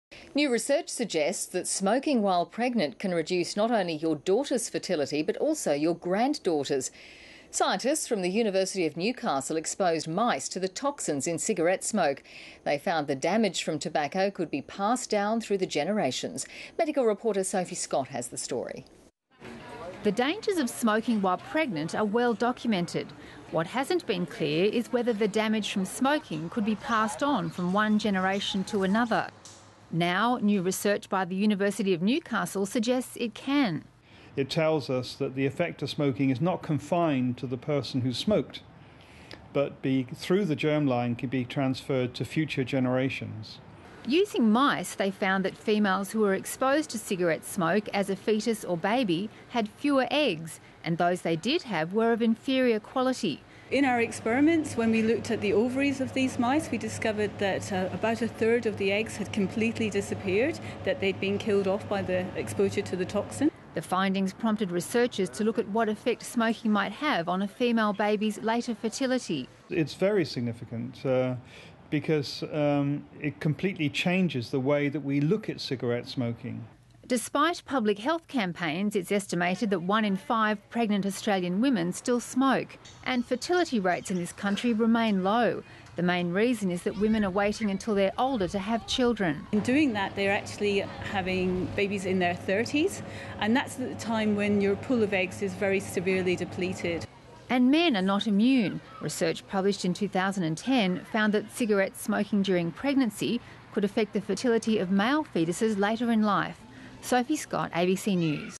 Research reveals smoking affects fertility
Scientists in Newcastle have found that smoking while pregnant can affect the fertility of unborn babies and that of future generations.